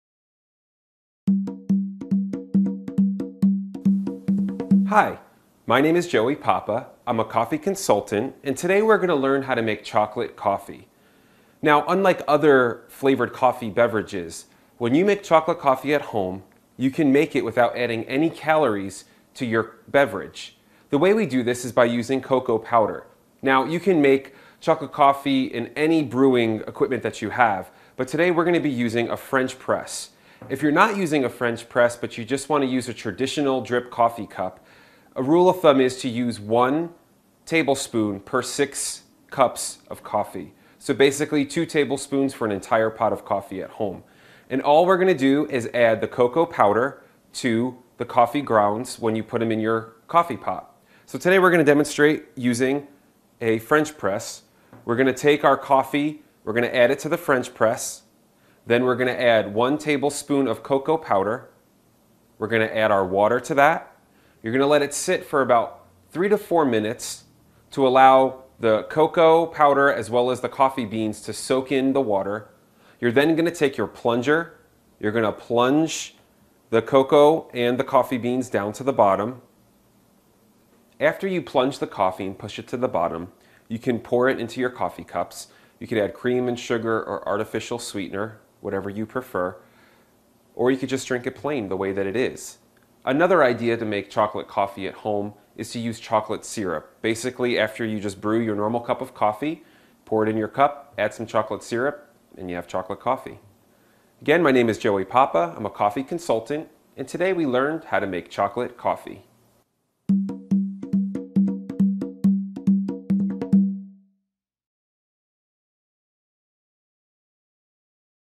How to Make Chocolate Coffee
How to Make Chocolate Coffee. Part of the series: Coffee Making. Making chocolate coffee is something that you can do right from the comfort of your own kitchen. Make chocolate coffee with help from a coffee industry professional in this free video clip.